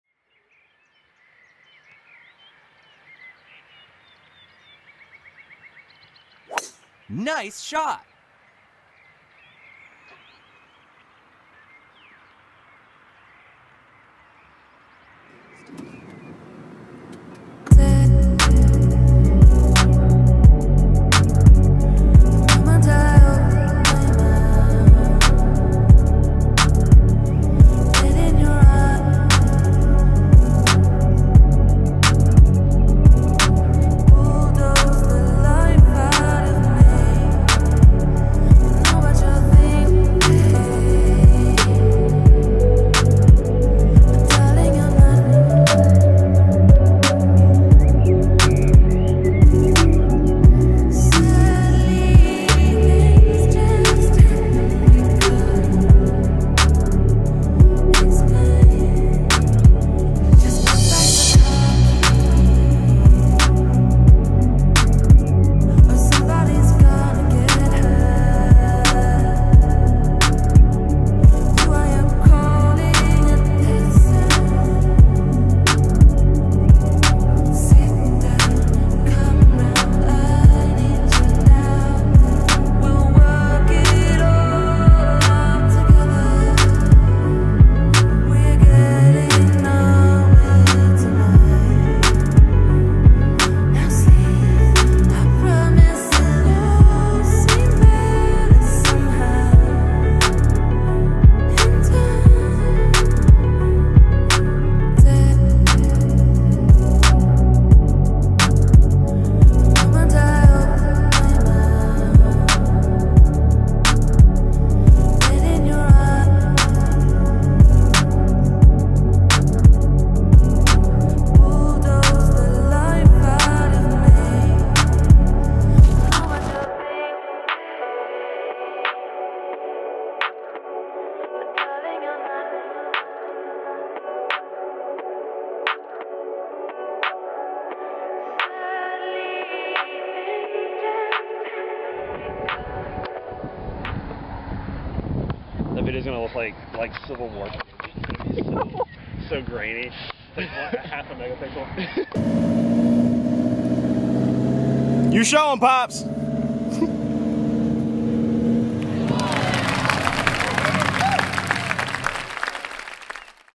Concours de Elegance 2023 | Supercar Heaven (4K)
Here's my 2023 Concours de Elegance Feature Film! Crazy show, amazing cars, good vibes. Really thankful for the opportunity to film this spectacular event and I'm excited for the next one.